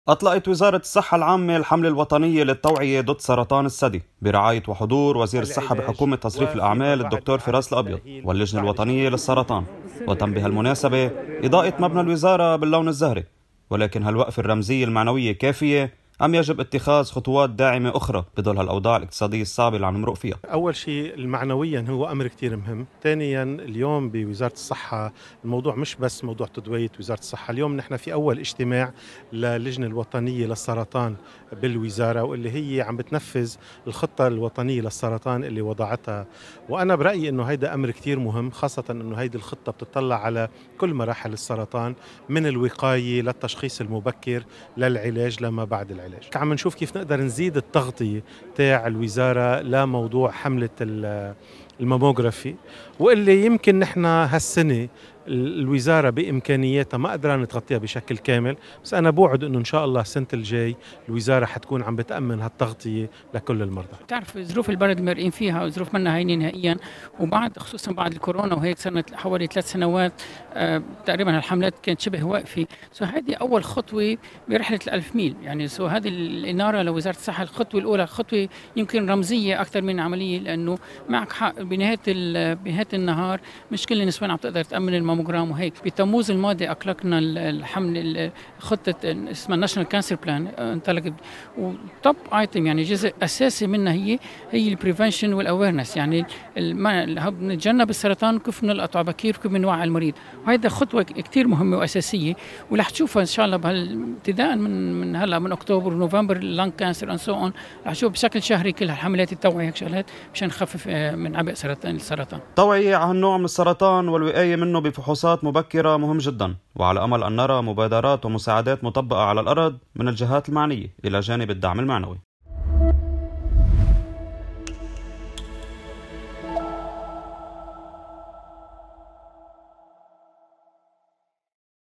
وزارة الصحة تضيء مبناها لأجل النساء.. "لمكافحة سرطان الثدي"!
في شهر التوعية حول سرطان الثدي، أضاءت وزارة الصحة مبناها باللون الزهري، فماذا قال الوزير لـ"هنا لبنان"؟ وما الاستراتيجية المتبعة في موضوع مرضى السرطان؟

تقرير: شربل حنينة